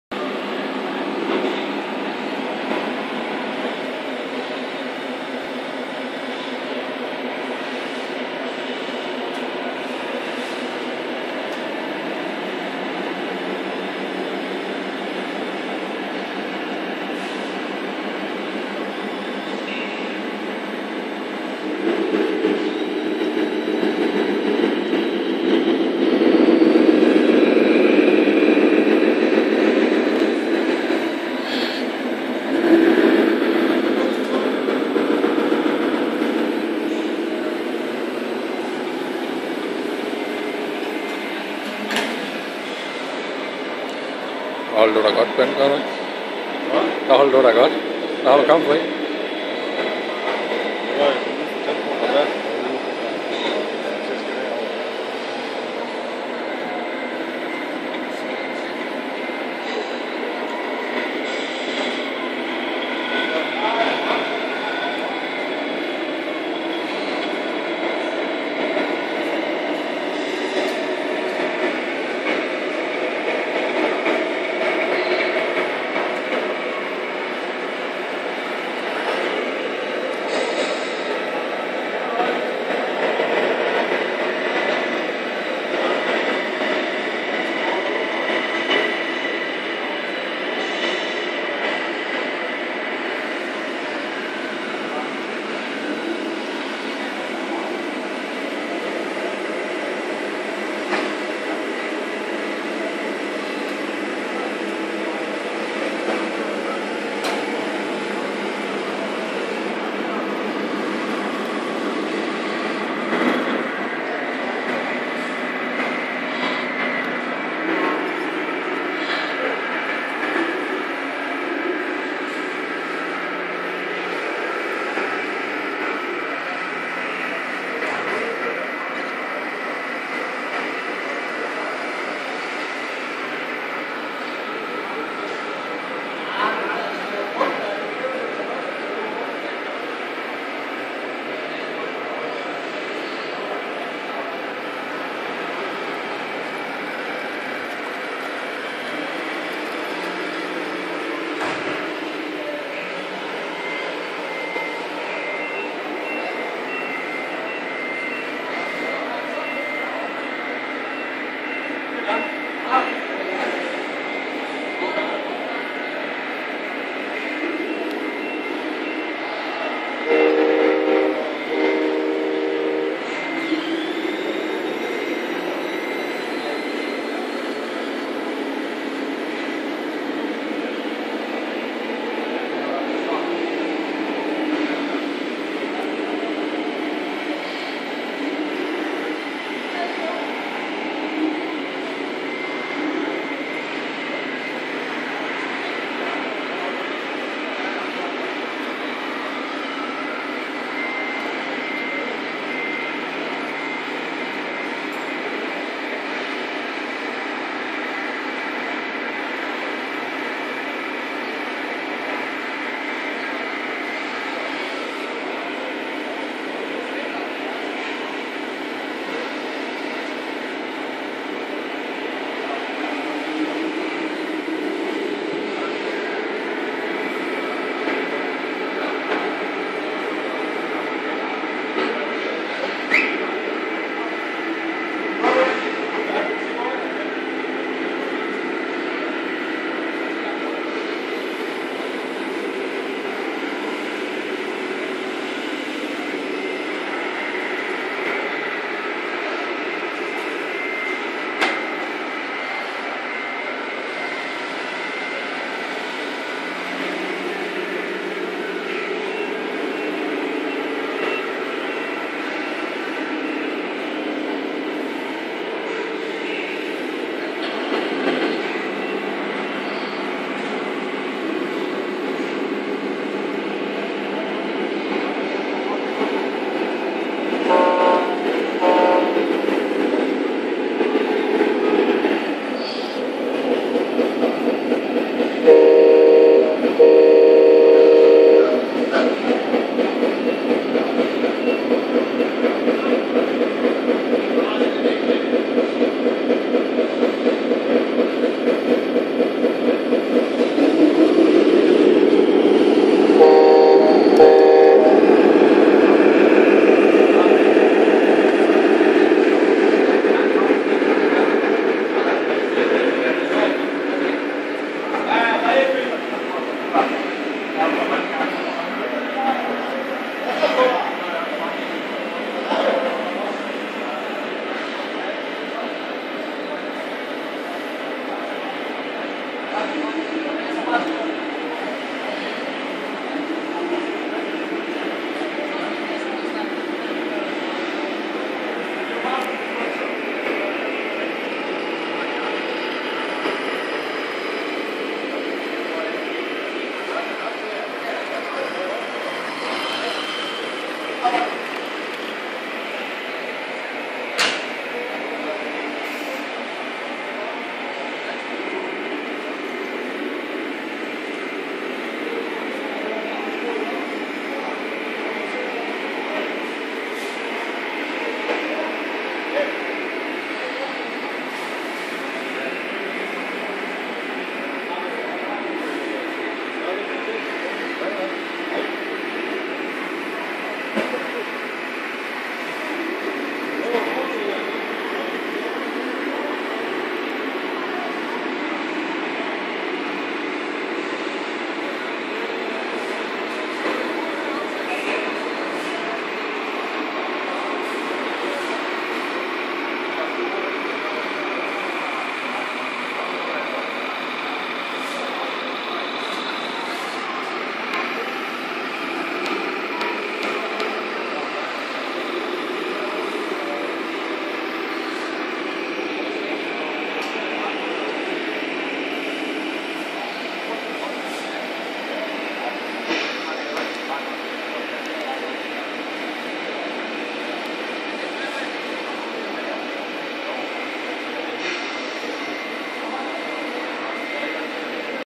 Rc Tamiya lorry/trucks Durup Denmark.
Truck meeting. Sorry for the shaking and the blur while zoomed.